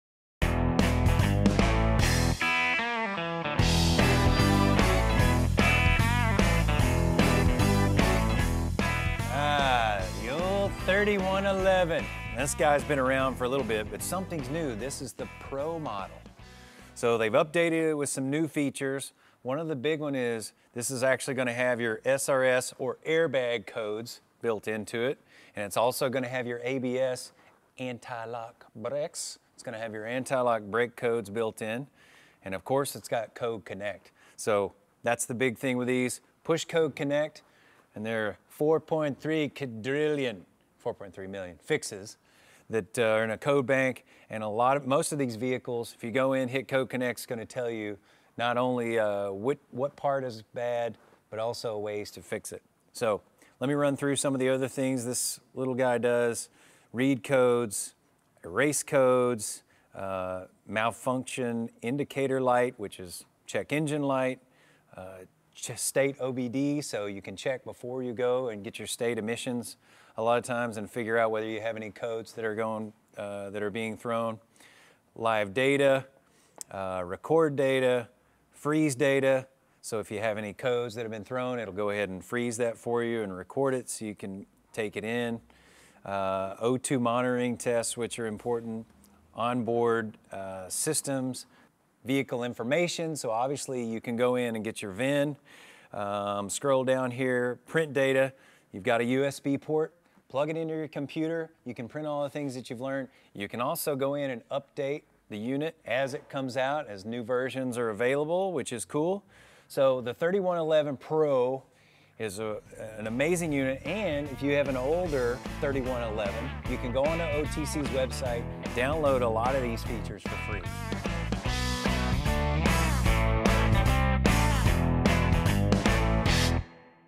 OTC 3111Pro
Bryan Fuller shows off the newly improved 3111Pro from OTC. Featuring expanded ABS & SRS coverage, as well as 4.3 million verified CodeConnect fixes, it's the best scan tool out there.